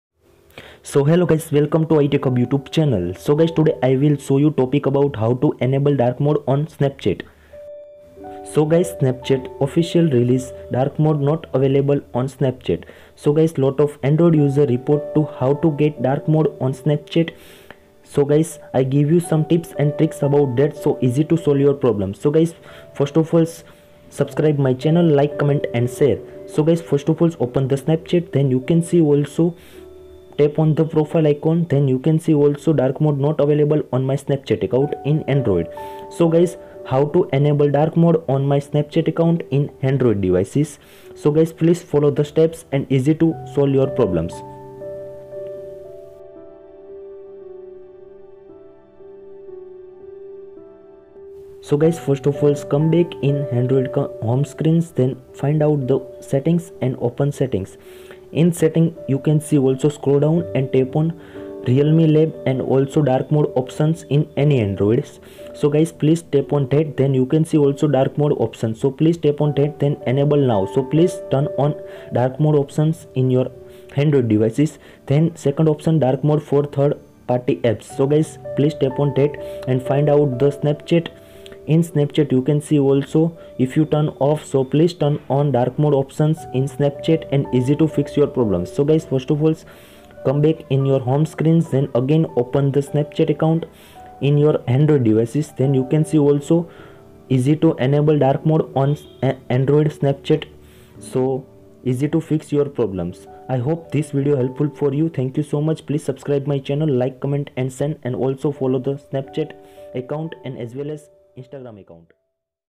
How To Enable Dark Mode On Snapchat [iPhone & Android] - Snapchat Dark Mode 2022
Snapchat Official Dark Mode Not Release But Still In Android Devices Enable Or Turn On Dark Mode On Snapchat.

I Give You Some Tips & Tricks Easy To Enable Dark Mode On Snapchat